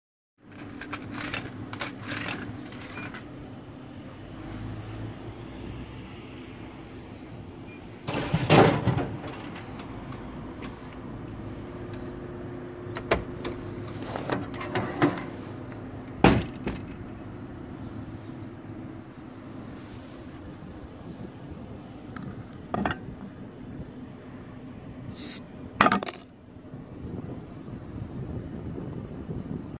昔懐かしの瓶コーラ自動販売機
厚木市温水にあります矢田商店にて撮影